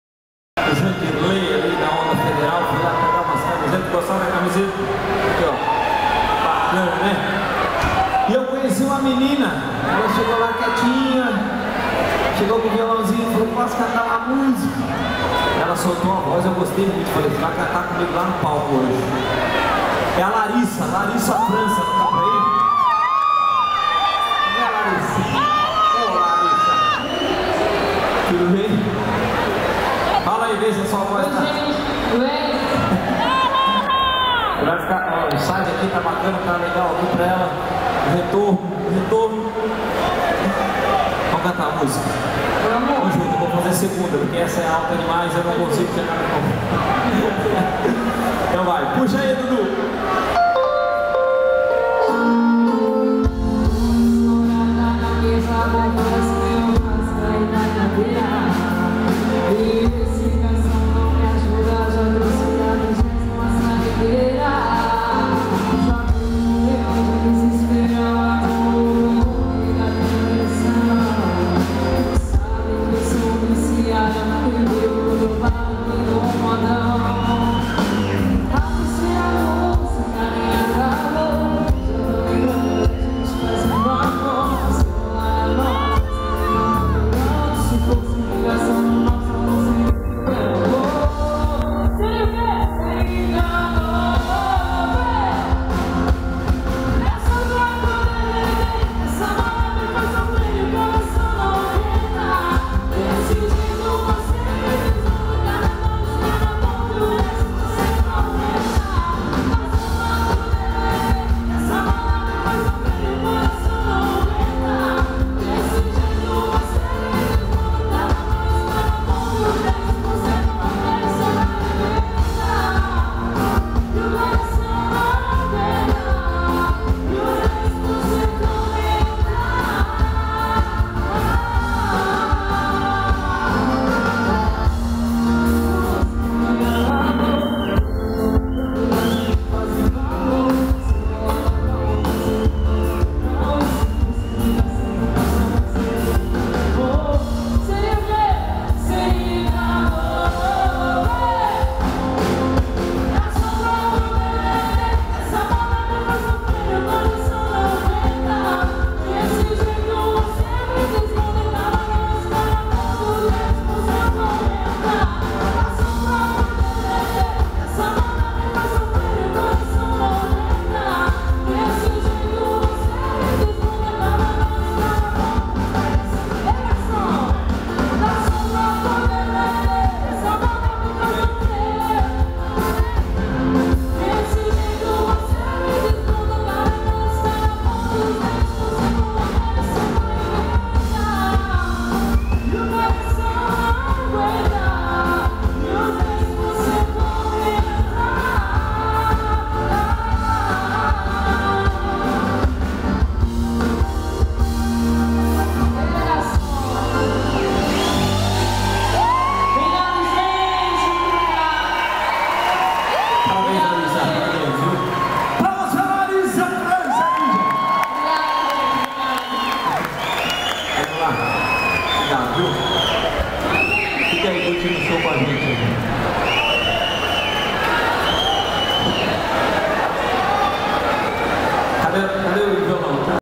Participação do show Fernando & Sorocaba| Larissa França
Sábado foi simplesmente mágico. Realizei meu sonho conhecer de pertinho Fernando & Sorocaba e principalmente subir no palco para cantar com o Fernando Zor. Obrigada a todos os envolvidos pelo carinho. O Show foi no Caramba´s em Indaial - Santa Catarina.